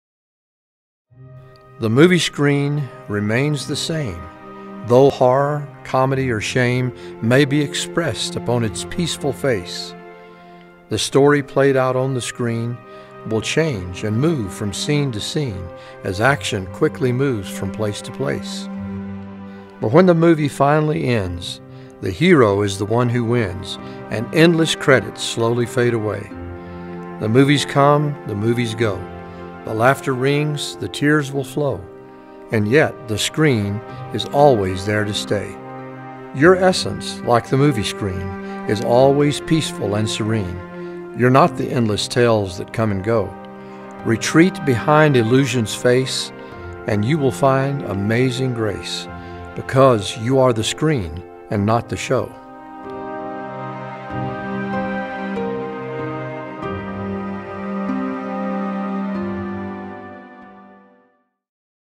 The Screen or The Movie
Darrell Scott reveals in this poem how just as a movie and the screen on which it shows differ, so are one's essence separate from the the constantly changing display of life.